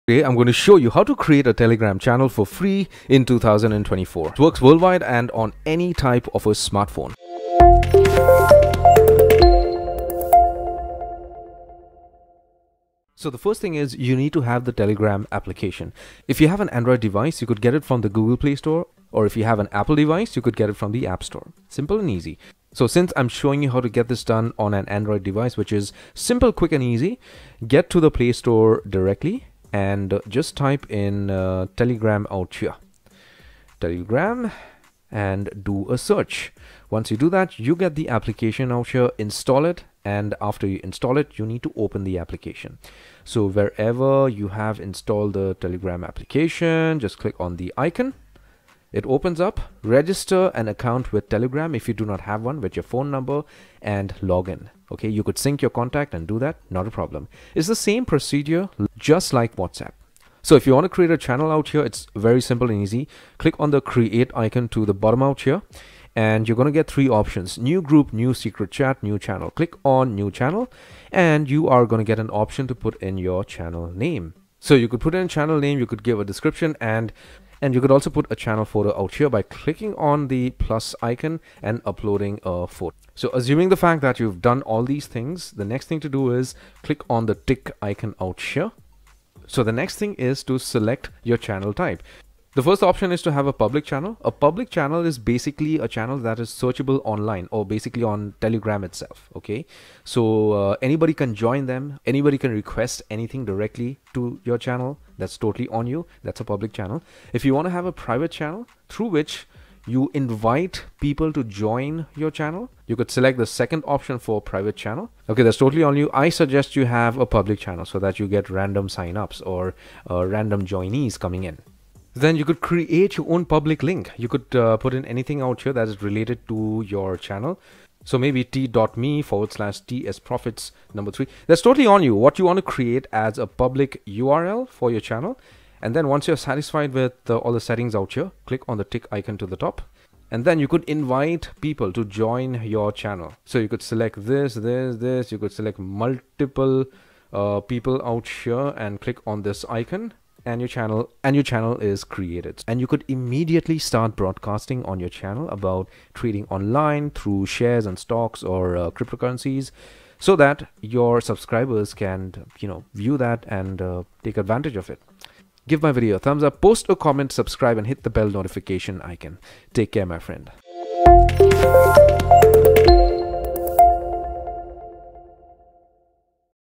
How to create telegram channel in 2024 | How To Create Public/Private Telegram Channel in 3 Minutes
Cheers!!!
Ken
Team CYGNATECH